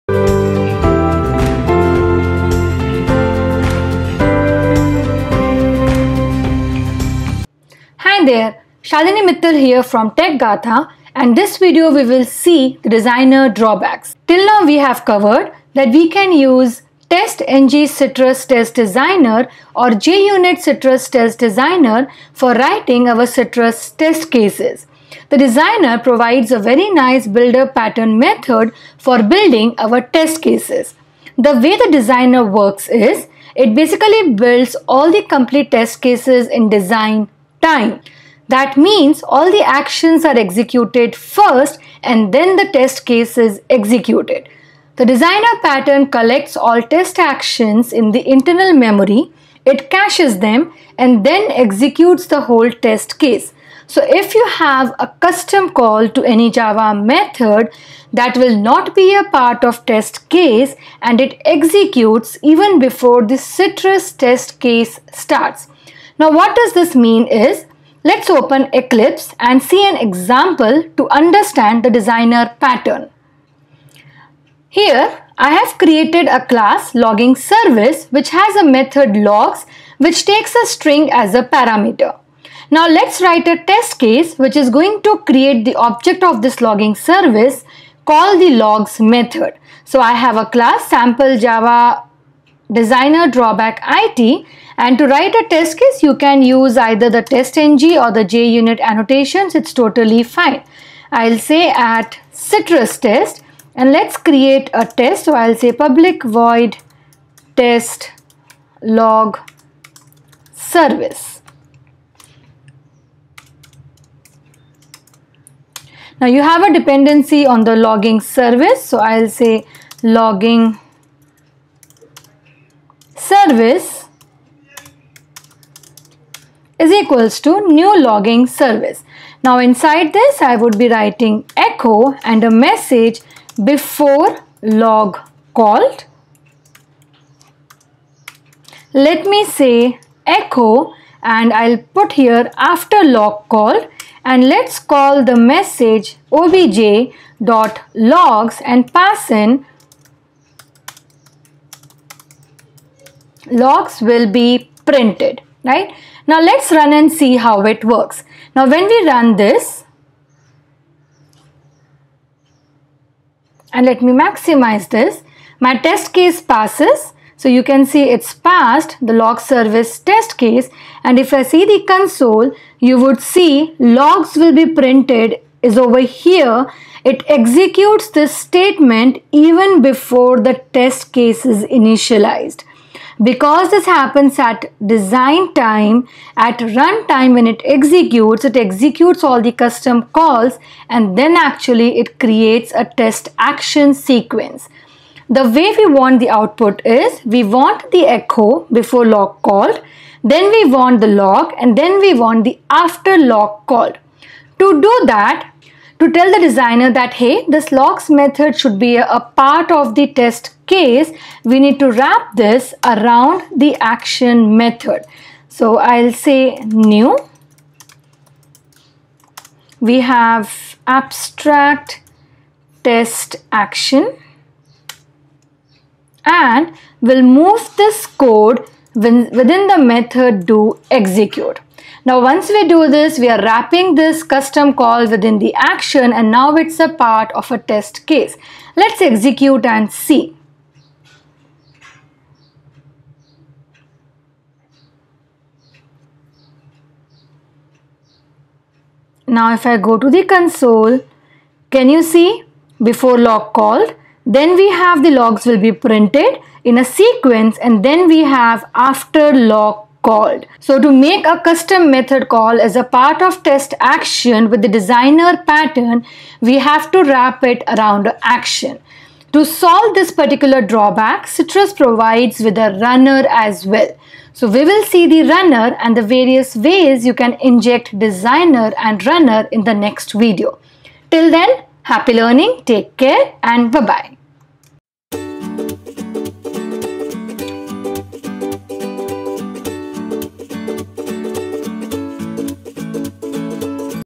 Citrus Integration Testing Framework 04 - Citrus TestDesigner Drawbacks
This video explains about the citrus designer drawbacks whether using testng or junit framework while mixing java code with  the citrus test cases.